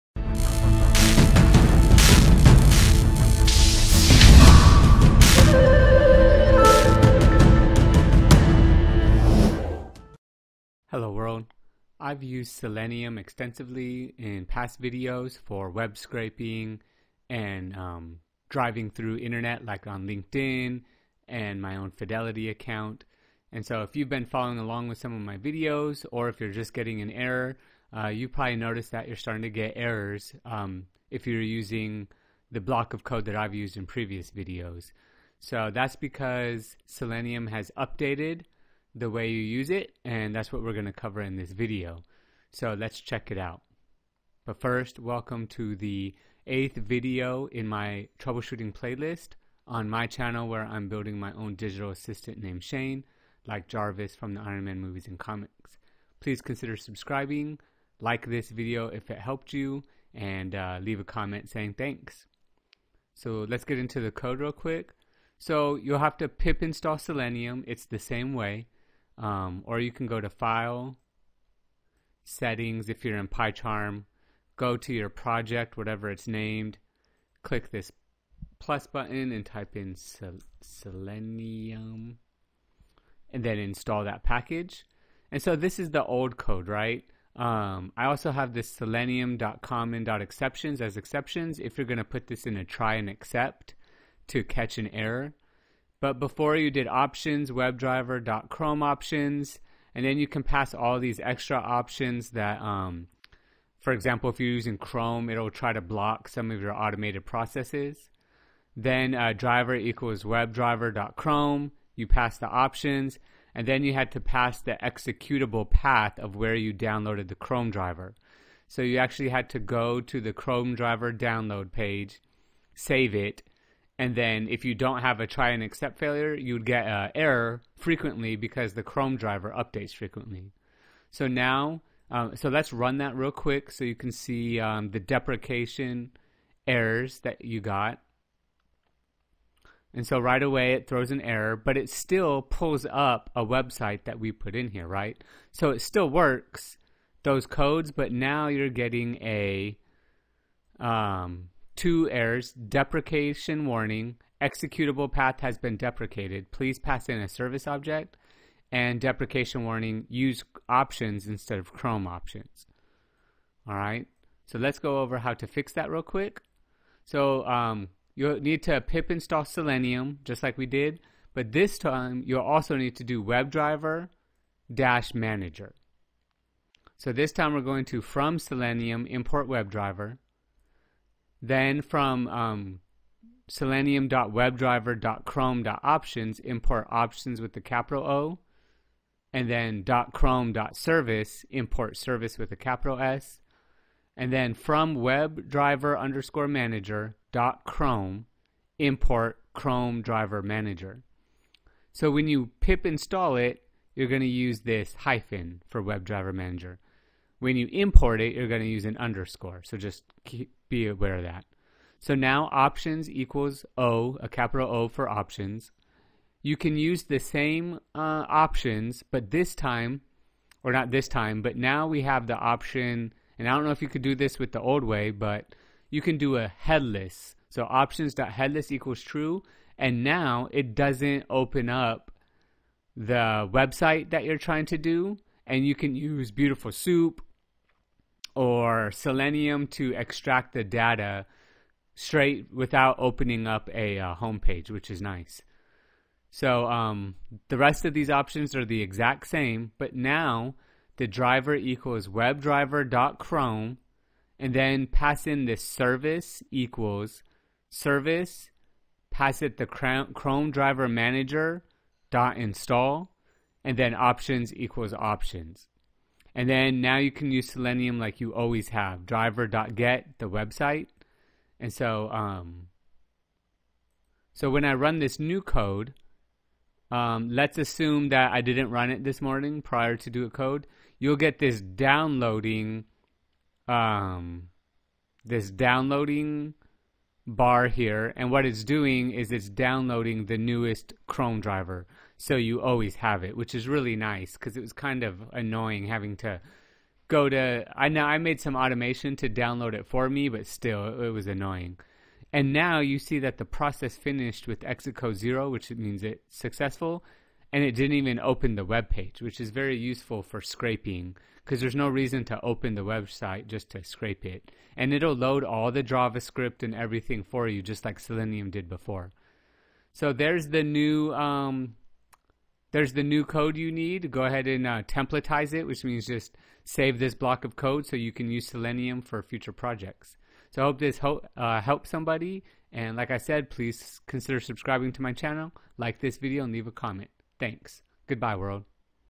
New Selenium Setup -- DeprecationWarning: executable_path has been deprecated (Troubleshooting #8)
I've used Selenium extensively on my channel for webscraping and automating internet processes. Selenium has been updated recently, so you may be getting these errors

- DeprecationWarning: executable_path has been deprecated, please pass in a Service object

- DeprecationWarning: use options instead of chrome_options

Check out this quick video on how to fix these errors and some advancements to Selenium.